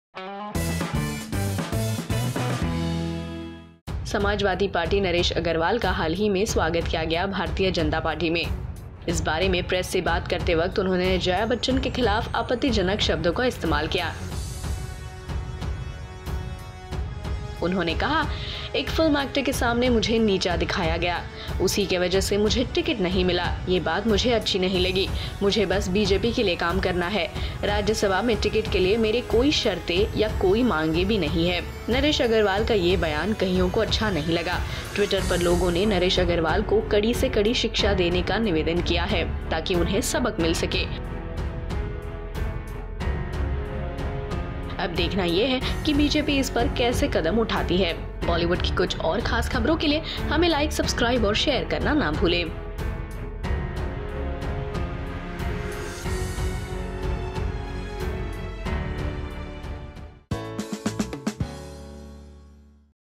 Jaya bachchan INSULTED By Naresh Agarwal (כתוביות בעברית)
Samajwadi party MP Naresh Agarwal was welcomed to the Bhartiya Janta Party. Talking to the press, Naresh Agarwal made an indecent remark towards Jaya Bachchan, he said
"My status was reduced to that of a film actor. Because of her, the ticket was not given to me, which I did not find right. I just want to serve the BJP, I have no conditions and no demand for a ticket in the Rajya Sabha."
His remark didn't go down well with many.  Twitterati demanded a stricter punishment and insisted the BJP government to sack him so that he teaches his lesson!
Let's wait and watch to see what kind of punishment with Mr. Naresh Agarwal get